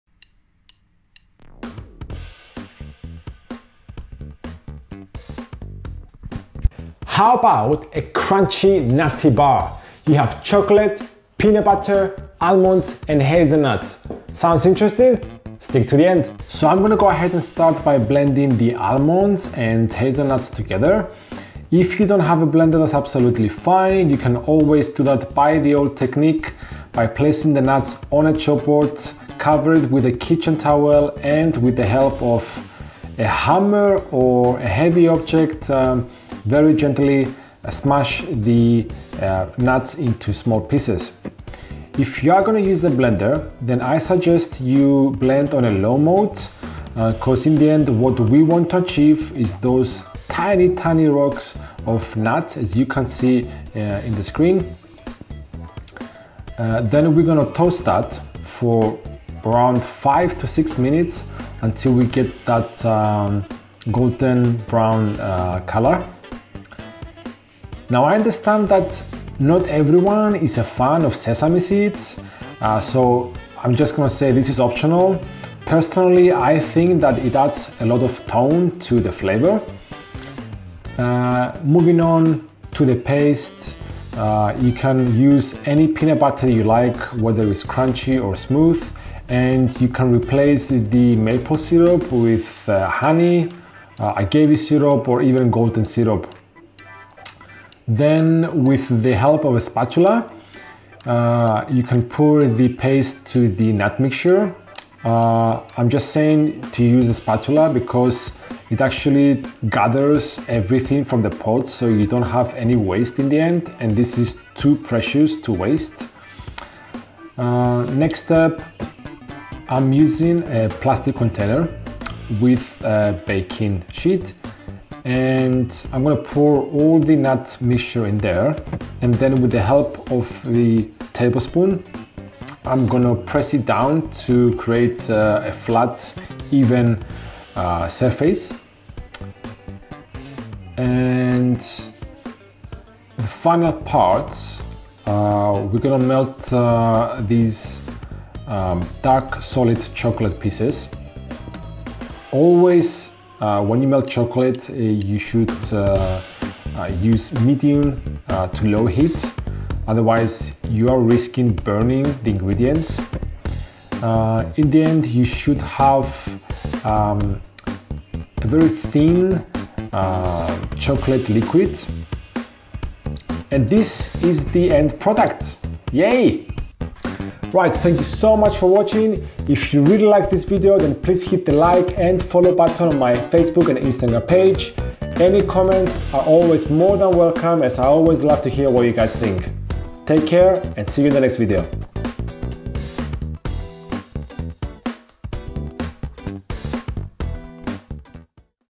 NUT BAR (PEANUT BUTTER & CHOCOLATE)
Ingredients

NUT MIX
120gr Almonds
120g Hazelnuts
40g Sesame seeds

PASTE
120gr Peanut Butter
3 tbs coconut oil
3 tbs maple syrup

Chocolate Glaze
100gr Solid dark chocolate
3 tbs coconut oil
3 tbs maple syrup